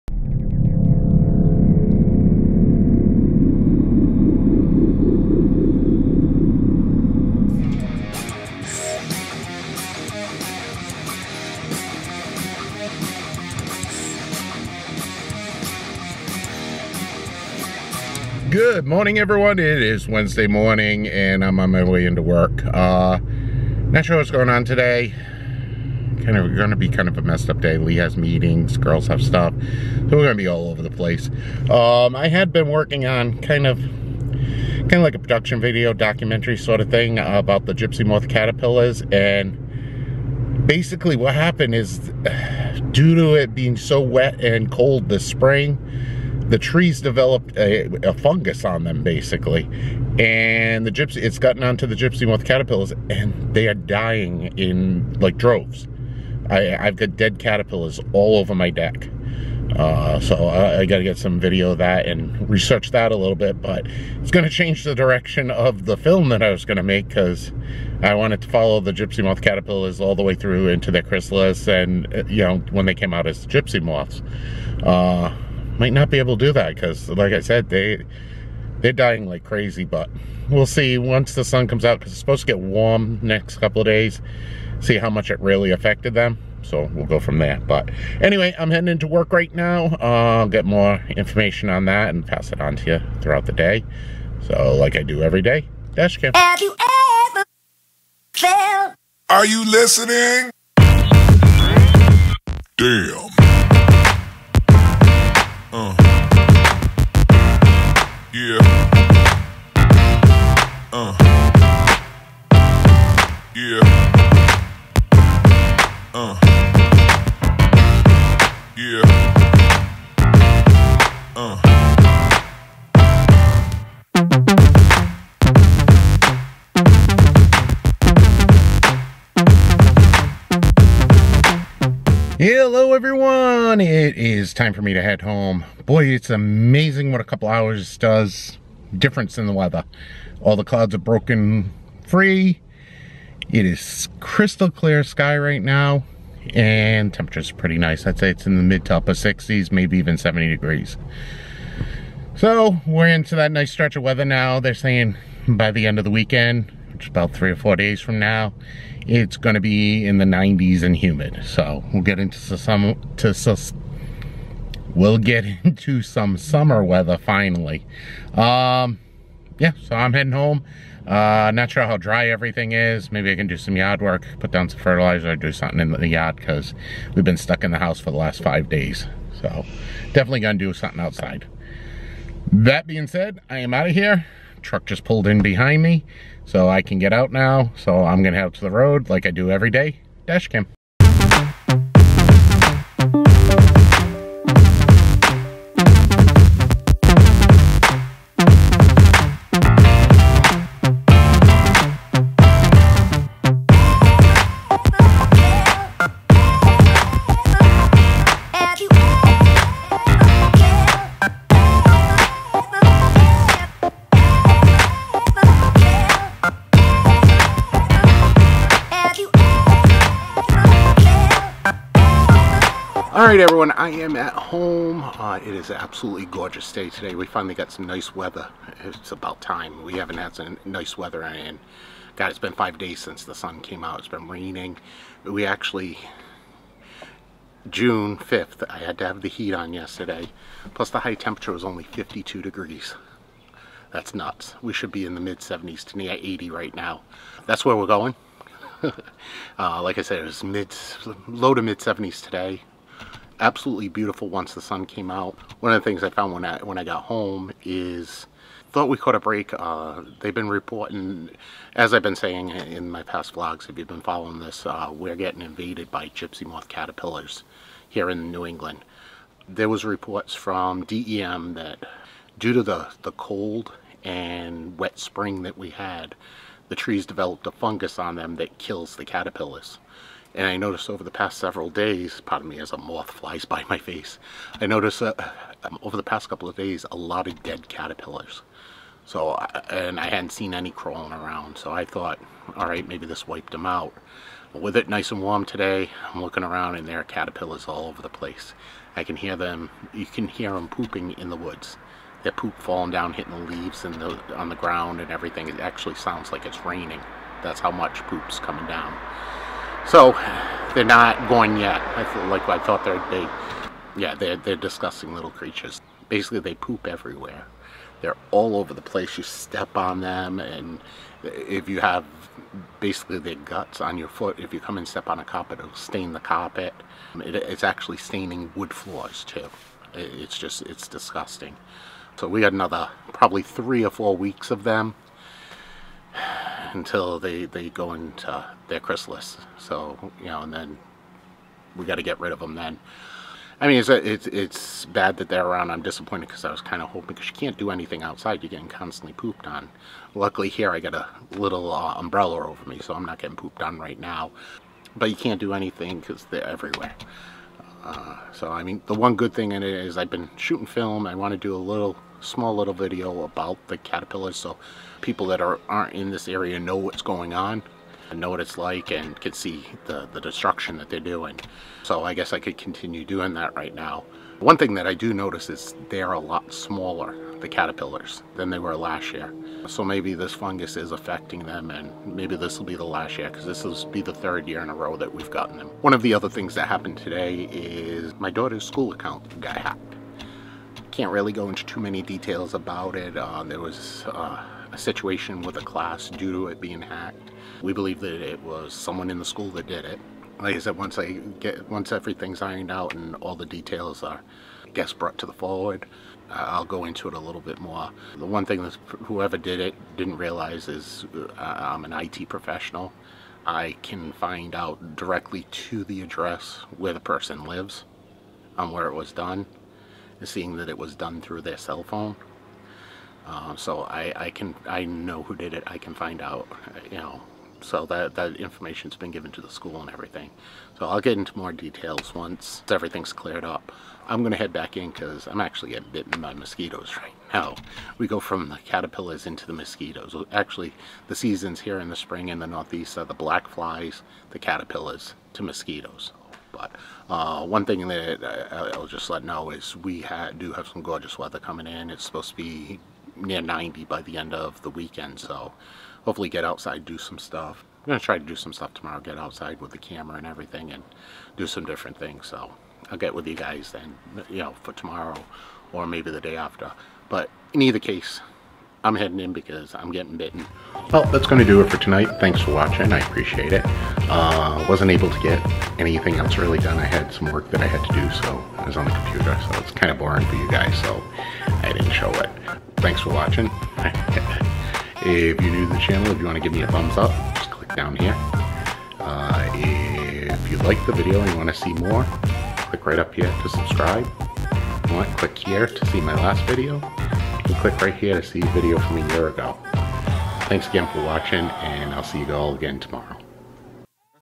They're Back (6.7.17) #897
After a couple of days reprieve we take on the full onslaught of the Gypsymoth Caterpillar Invasion of 2017.